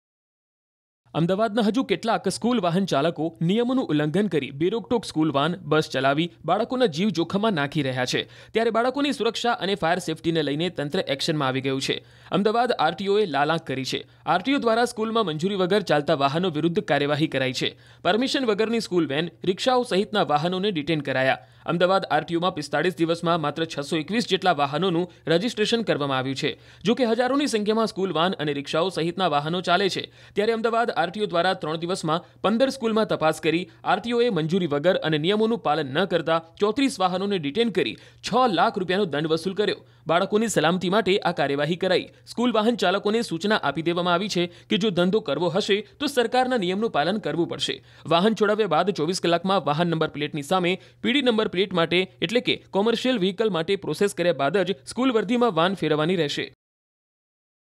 Ahmedabad News: મંજૂરી વિનાની સ્કૂલ વાનને 6 લાખનો દંડ ! | School van | RTO | N18V
Still some school vehicle drivers of Ahmedabad are endangering the lives of children by driving unstoppable school vans and buses in violation of the rules.
અમદાવાદના હજુ કેટલાક સ્કૂલ વાહન ચાલકો નિયમોનું ઉલ્લંઘન કરી  બેરોકટોક  સ્કૂલ વાન, બસ ચલાવી બાળકોના જીવ જોખમમાં નાંખી રહ્યા છે.n18V